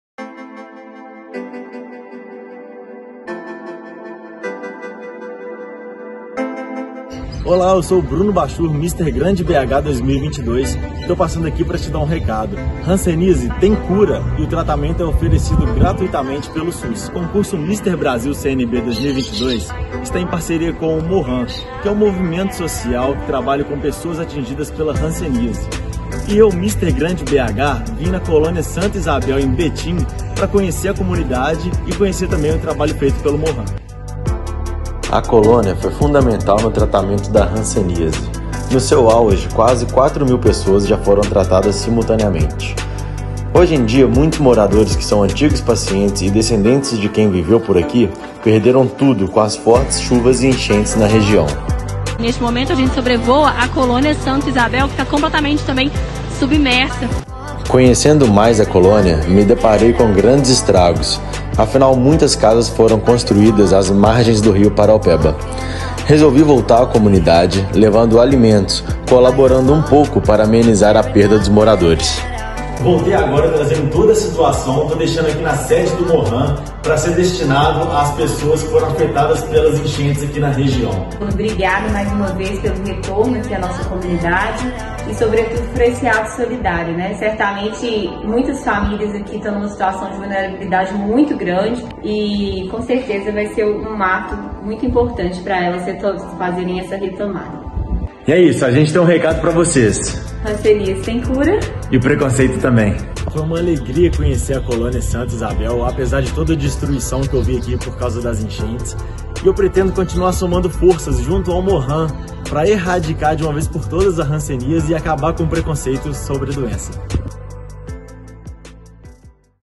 Mister Brasil CNB 2022 - From the Ground Up - Mister Grande BH CNB
Bruno Bachur, Mister Grande BH CNB 2022, estará no Concurso Nacional de Beleza 2022, concurso que elegerá o Mister Brasil CNB 2022 e que acontecerá dia 07 de maio, diretamente do Hotel Sibara, em Balneário Camboriú (SC), com transmissão ao vivo pelo youtube (TV CNB).